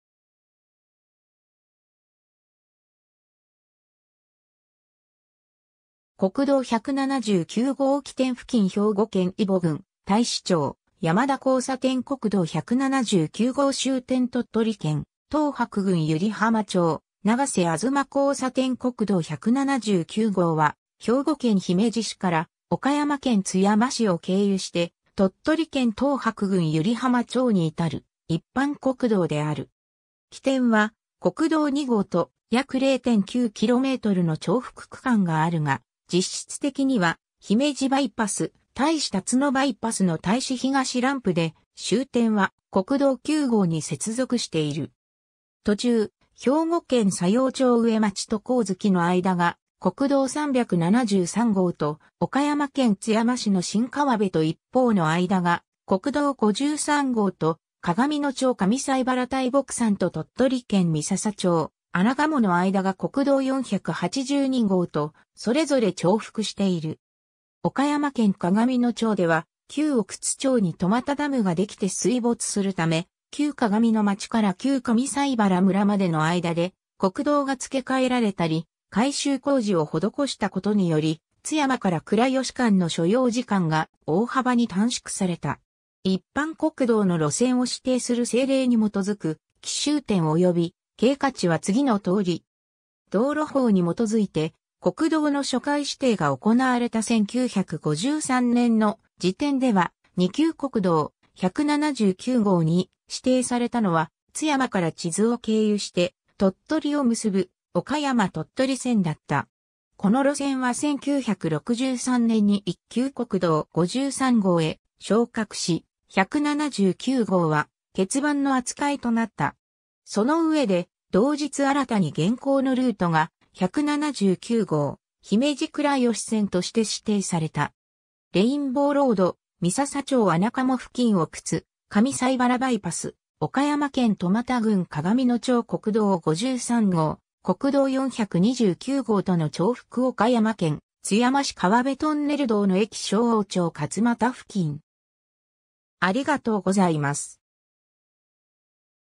国道179号
国道179号　起点付近 兵庫県揖保郡太子町　山田交差点 (国道2号との交差点) 国道179号　終点 鳥取県東伯郡湯梨浜町 長瀬東交差点 国道179号（こくどう179ごう）は、兵庫県姫路市から岡山県津山市を経由して、鳥取県東伯郡湯梨浜町に至る一般国道である。
 起点は国道2号と約0.9 kmの重複区間があるが、実質的には姫路バイパス、太子竜野バイパスの太子東ランプで、終点は国道9号に接続している。
途中、兵庫県佐用町上町と上月の間が国道373号と、岡山県津山市の新河辺と一方の間が国道53号と、鏡野町上齋原大木山と鳥取県三朝町穴鴨の間が国道482号と、それぞれ重複している。
岡山県鏡野町では、旧奥津町に苫田ダムができて水没するため旧鏡野町から旧上齋原村までの間で国道が付け替えられたり、改修工事を施したことにより、津山から倉吉間の所要時間が大幅に短縮された。
 道路法（昭和27年法律第180号）に基づいて、国道の初回指定が行われた1953年（昭和28年）の時点では、二級国道「179号」に指定されたのは津山から智頭を経由して鳥取を結ぶ「岡山鳥取線」（現在の国道53号に相当）だった。
この路線は1963年（昭和38年）に一級国道53号へ昇格し、「179号」は欠番の扱いとなった。
 そのうえで、同日新たに現行のルートが「179号」「姫路倉吉線」として指定された。
 レインボーロード 三朝町穴鴨付近 奥津―上斎原バイパス 岡山県苫田郡鏡野町 国道53号・国道429号との重複 岡山県津山市河辺 トンネル 道の駅 勝央町勝間田付近 （2013年9月）